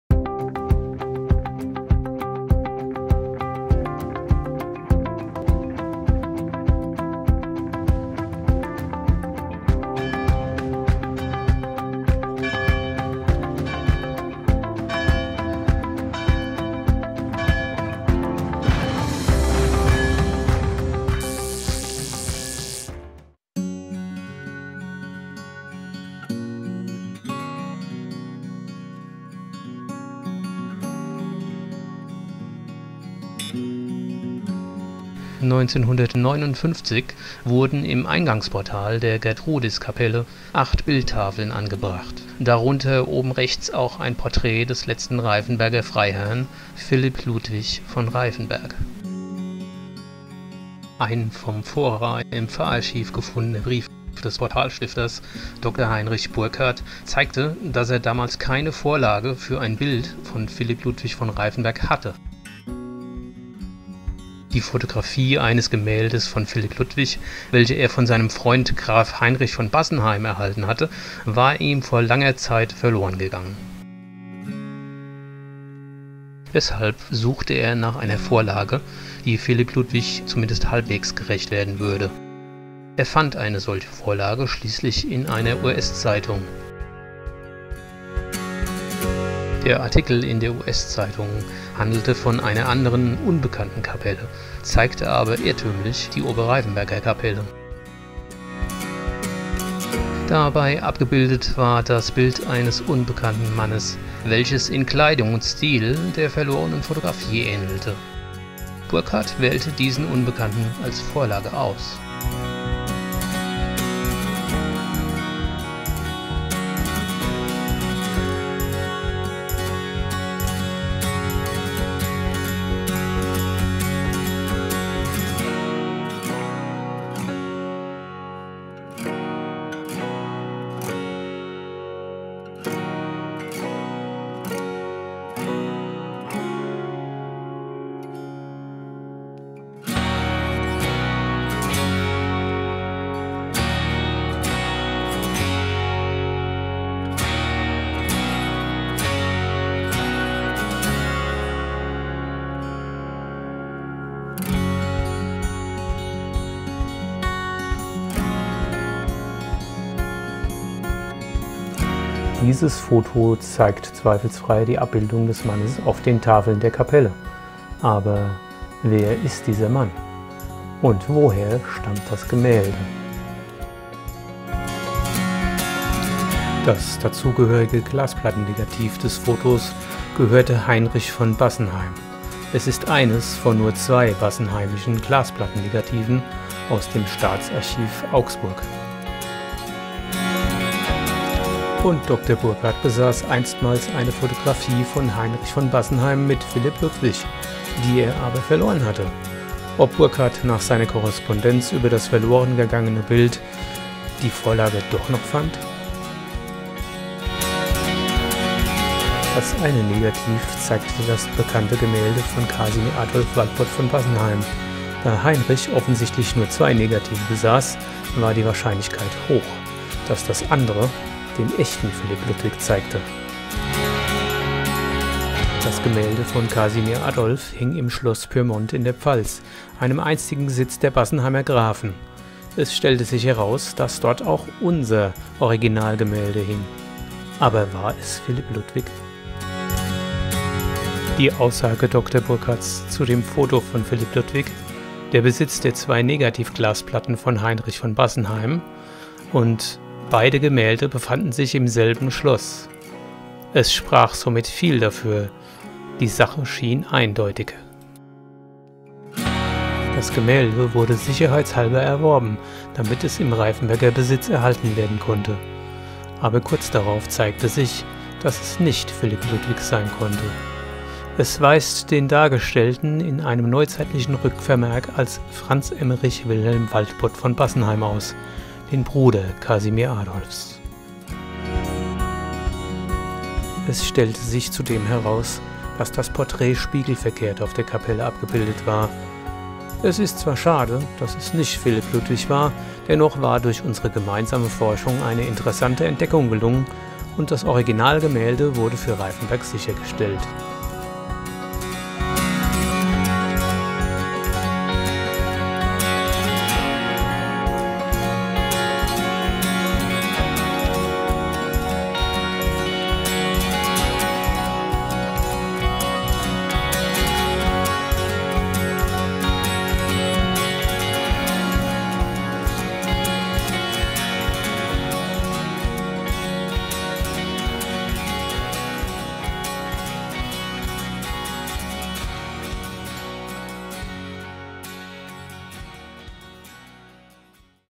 "Philipp Ludwig von Reiffenberg" auf dem Portal der Kapelle
Ist die abgebildete Person auf dem Eingangsportal der Kapelle Philipp Ludwig von Reiffenberg oder jemand anderes? Dr. Burkard, der das Portal 1959 stiftete, hatte zwar einstmals eine Fotografie von Philipp Ludwig von seinem Studienfreund Heinrich von Bassenheim erhalten. Aber diese hatte er vor längerer Zeit verloren. Also mußte ein anderes, zumindest ähnliches Portrait gefunden werden. Wie er selbst schrieb, fand er auch eines in einer US-Zeitung der ihm zumindests im Stil ähnlich sah. Aber verwendete er es auch? Ist auf der Kapelle Philipp Ludwig abgebildet oder nicht? Bei Nachforschungen von Dr. Heinz-Peter Mielke mit Unterstützung des Pfarr- und Ortsarchiv Reifenberg ergab sich ein wechelndes Bild der Lage.

Und noch eine Frage wird hiermit endlich beantwortet: Warum ziert das Anlitz von Philipp Ludwig auf PLE seit Ende 2021 ein kleiner Oberlippenbart?

Diese spannenden Fragen beantwortet das neue Video von PFORAtv!